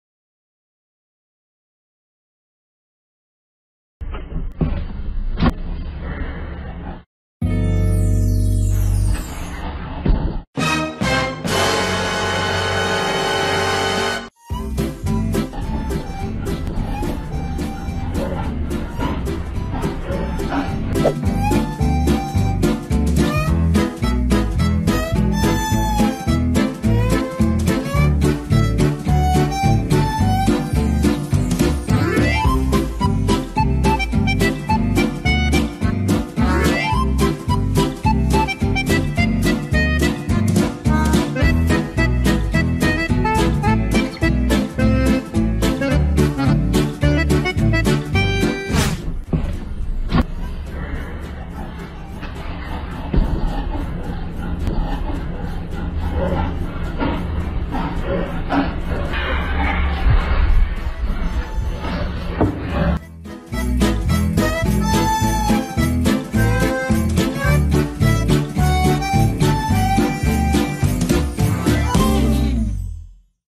พ่อครับผมพาเพื่อนมาเที่ยวบ้านครับ
เรื่องราวสุดฮา เมื่อหมาลูกรัก พาเพื่อนมาเที่ยวบ้าน แต่เดี๋ยวนะ เพื่อนเราหน้าตาคุ้นๆ